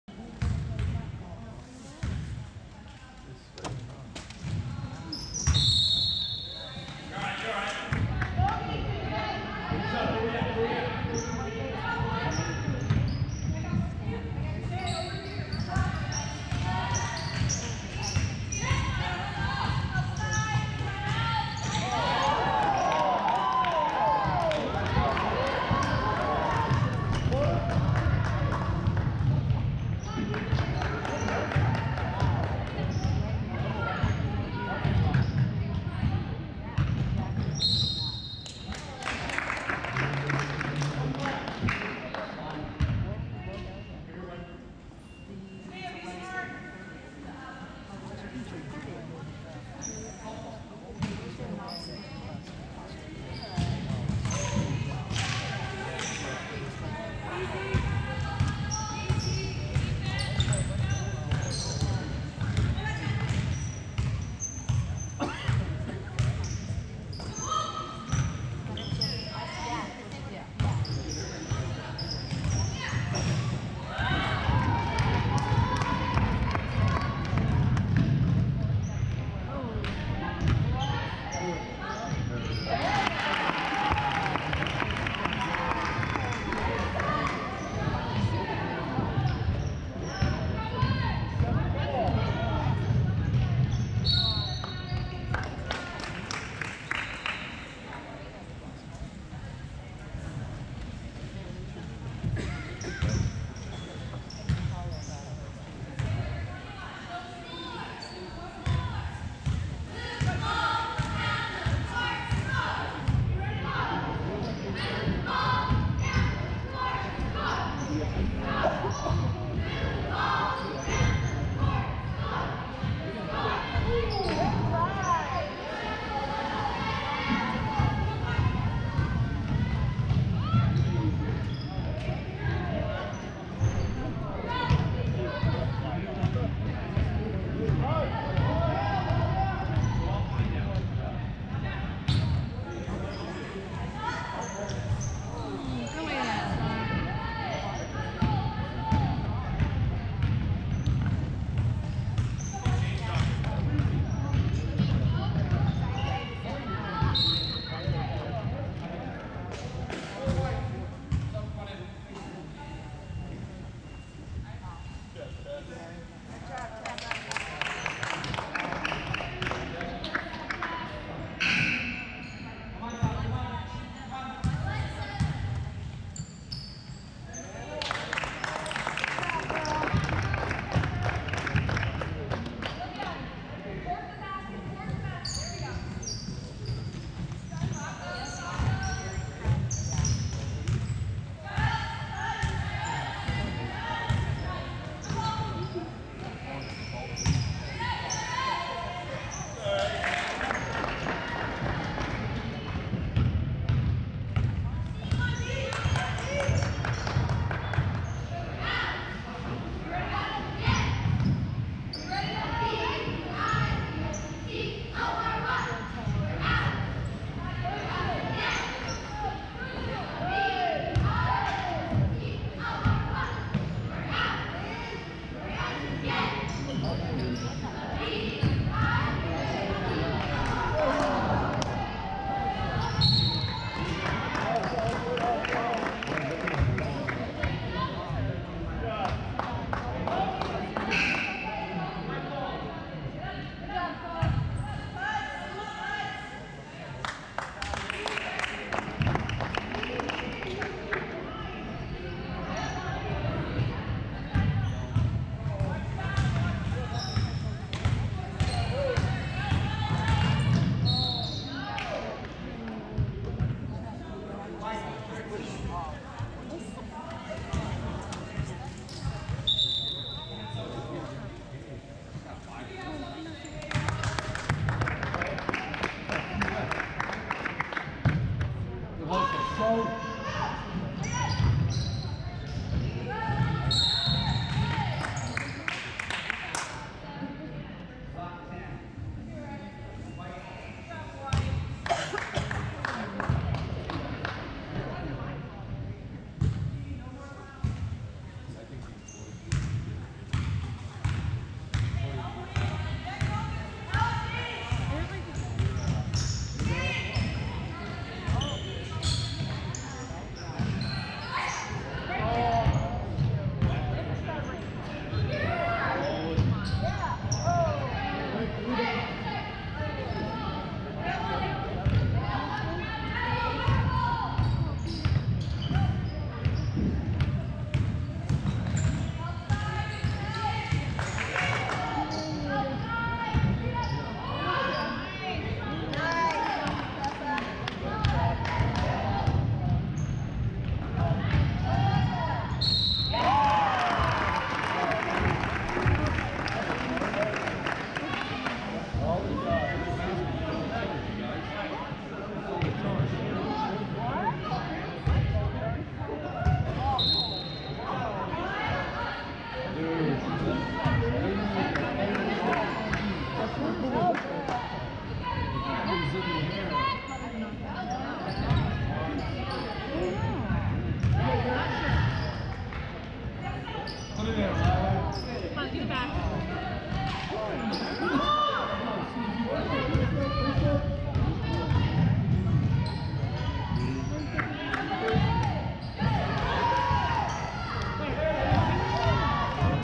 Papillion/LaVista South Girls Basket Ball Game - 1/7/19
Olivia playing basketball last night against Bellevue West (#5 white jersey). She had a great block at the very beginning and got the win. Go Papillion/LaVista South Titans! 6-3 on the year so far.